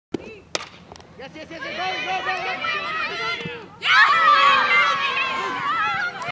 michelle reynolds diving catch
final out in National softball quarterfinal